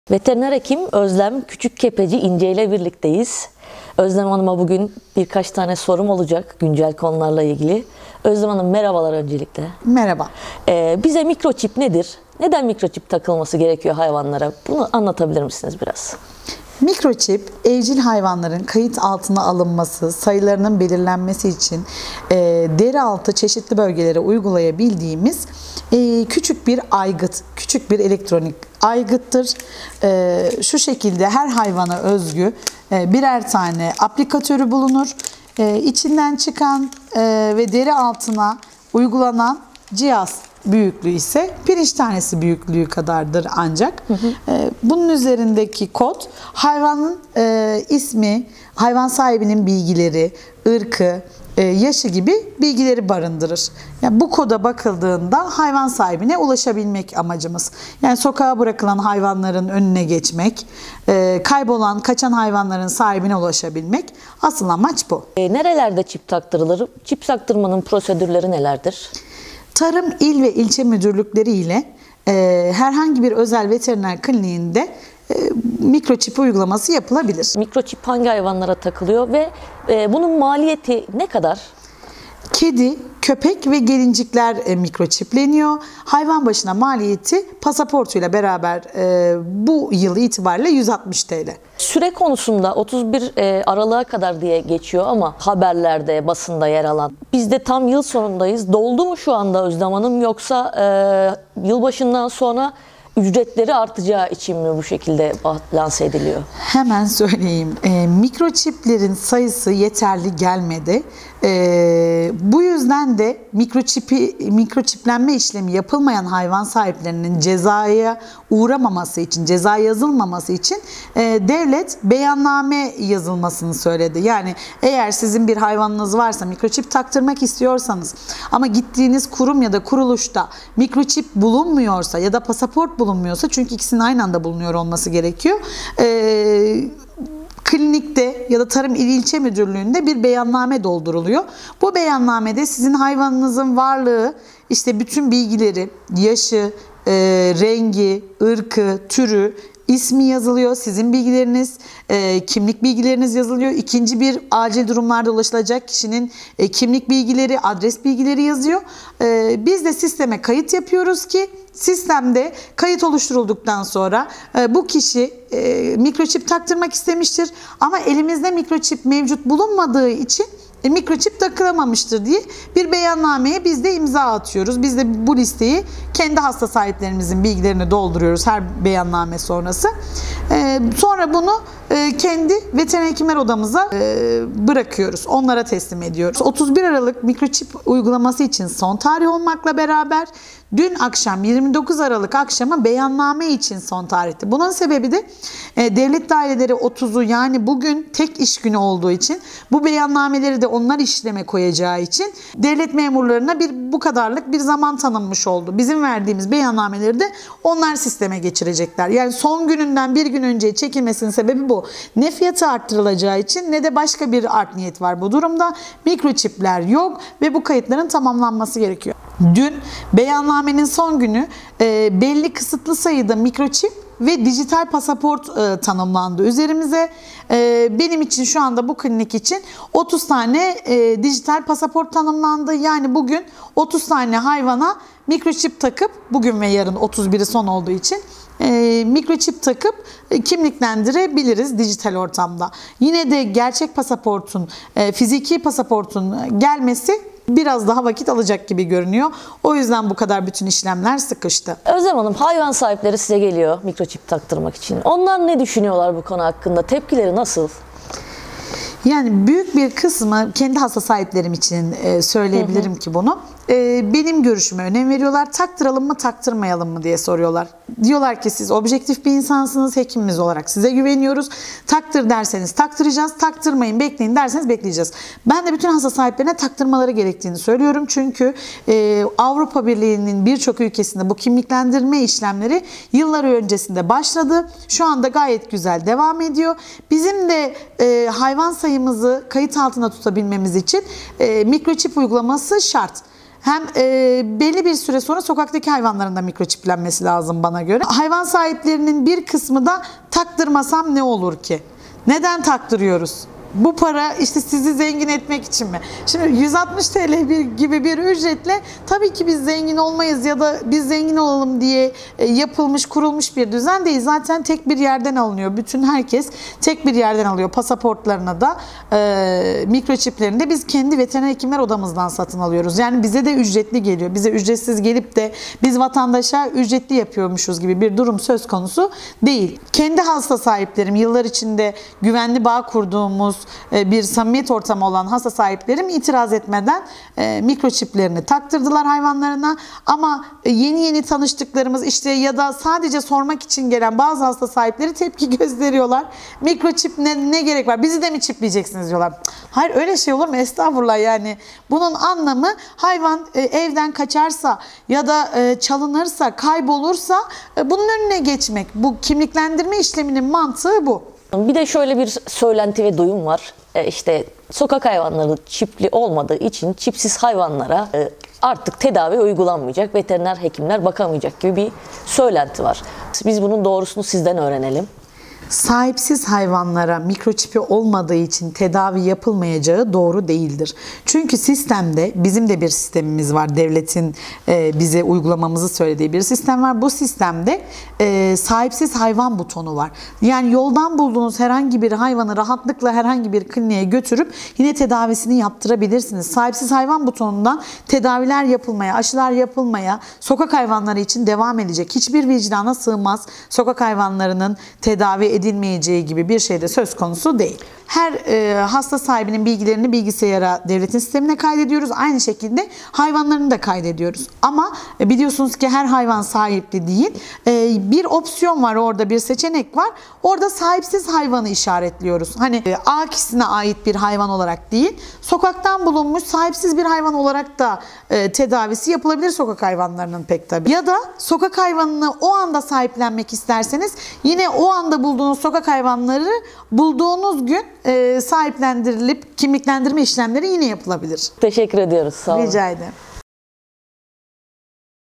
Mikroçip nedir, maliyeti ne kadar? Veteriner Hekim Özlem Küçükkepeci İnce Egeyön Pati'ye anlattı!
Veteriner Hekim Özlem Küçükkepeci İnce, Egeyön Pati'ye konuştu. Gündemde olan çip konusunu detaylarıyla değerlendirdi.